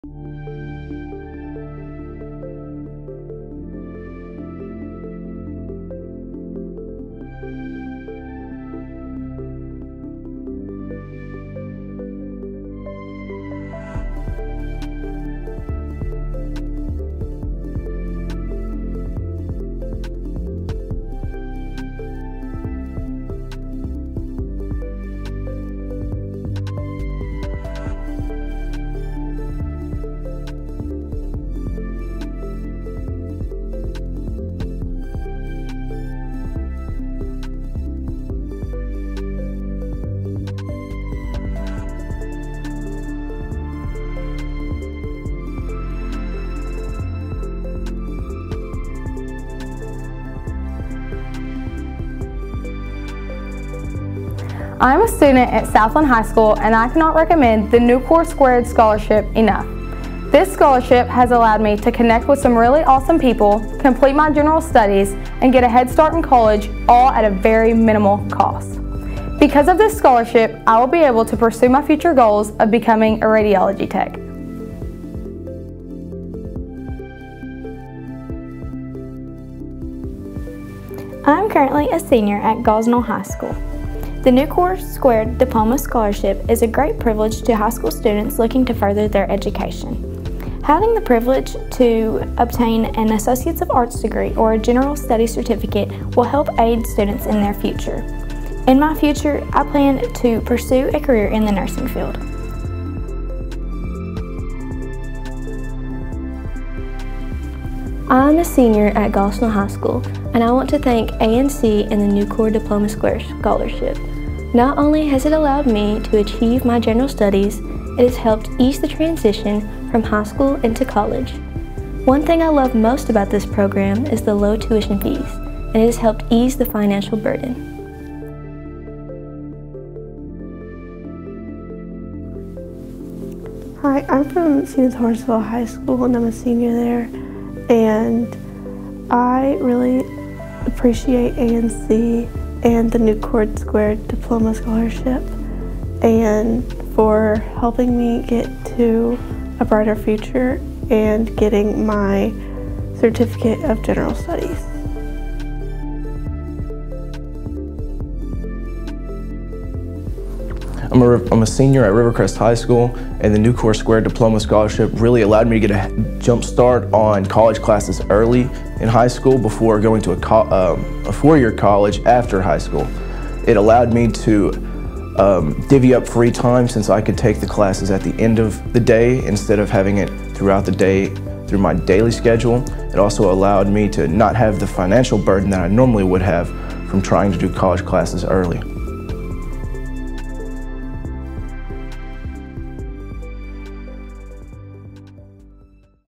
2024 Nucor Diploma² Program Recognition Video
We appreciate our community partners. Thank you, Nucor, for supporting our institution and our students!